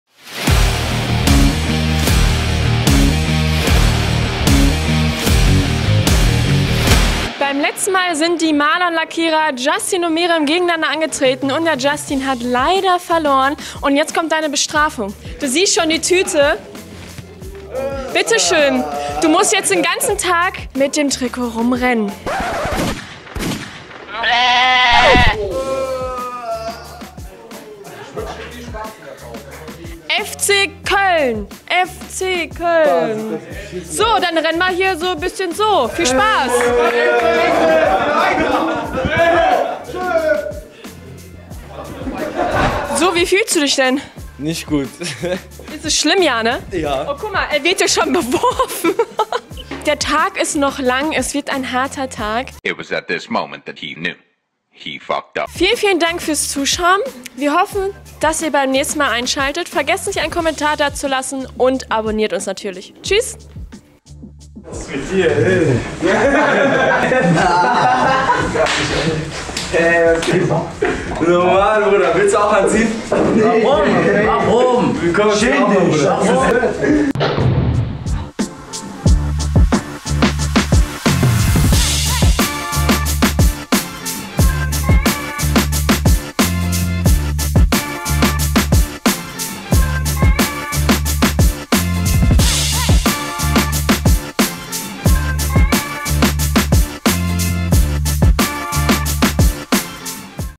Maler und Lackierer | Bestrafung #2 | Die Handwerk Challenge
Weiter geht's mit der Bestrafung.
Azubi Justin hat gegen Miriam verloren und muss nun einen "Walk of Shame" auf sich nehmen!

Die HandwerkChallenge: Jeden Donnerstag um 15:00 Uhr.